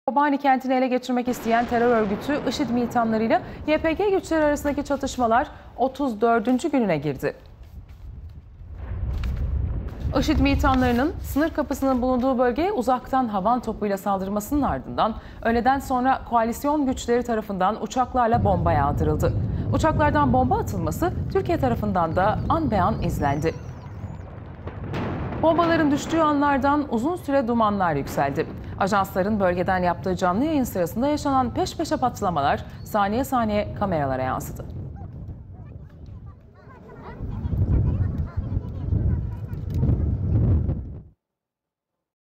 KOALİSYON GÜÇLERİNİN IŞİD'İ BOMBALAMA ANI KAMERADA
Kobani'de terör örgütü IŞİD ve YPG militanları arasındaki çatışmalar tüm şiddeti ile devam ediyor. Bir yandan da ABD öncülüğündeki koalisyon güçleri ŞİD hedeflerini vurmaya devam ediyor. Gelen son görüntülerde IŞİD hedeflerinin vurulma anları kameralara böyle yansıdı.